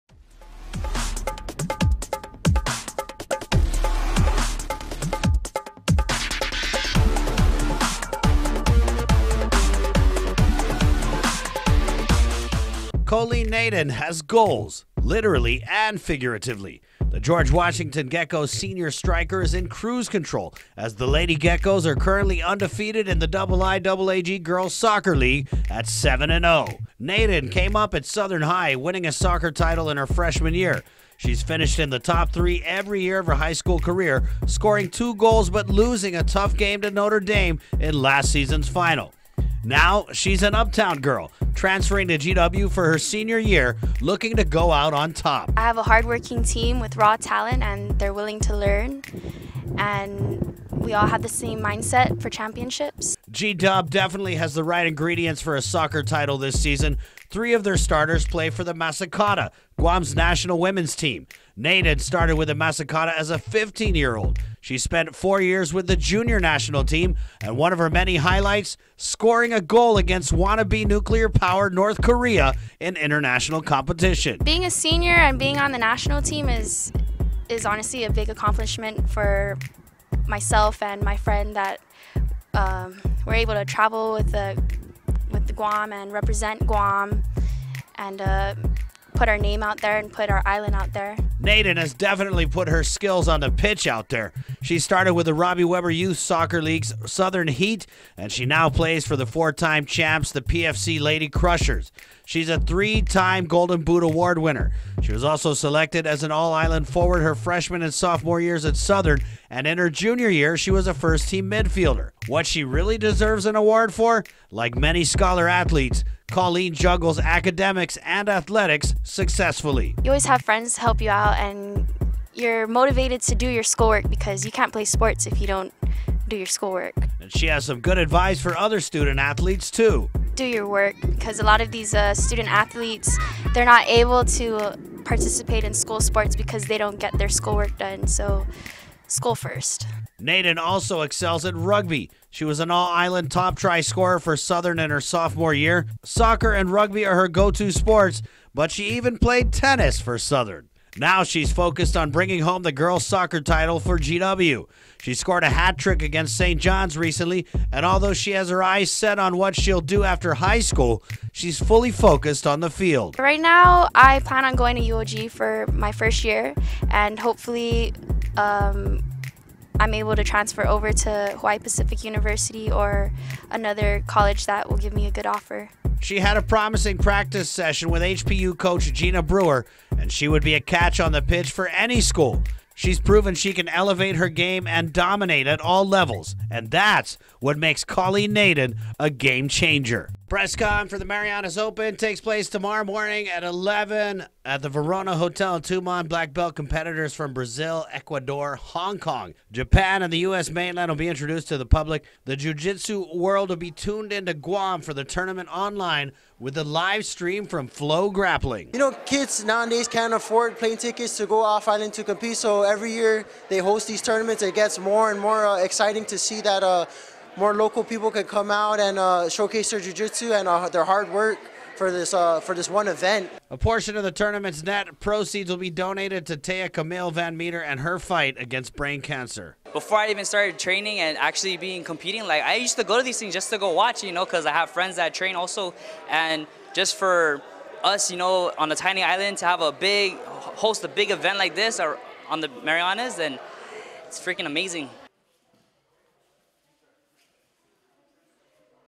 Gamechangers - GW's Colleen Naden has Geckos aiming for perfection
Colleen Naden has goals - literally and figuratively. The George Washington Geckos senior striker is in cruise control as the Lady Geckos are currently undefeated in the IIAAG girls' soccer league at 7-0.